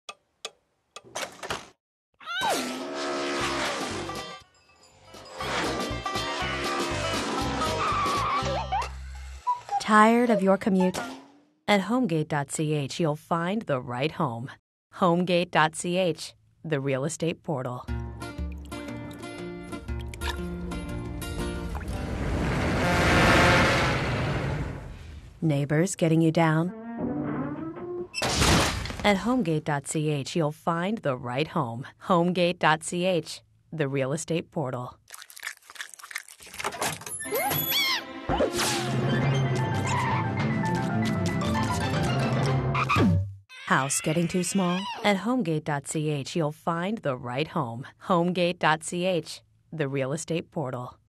Homegate.ch Spots
Neighbors getting you down?  Need a bigger house?  Tired of your commute? Homegate's online database of homes and apartments can help. 2009 Broadcast Campaign ADDY winner.

Client: Homegate.ch
Agency/Production House: Heisskiss/Boutiq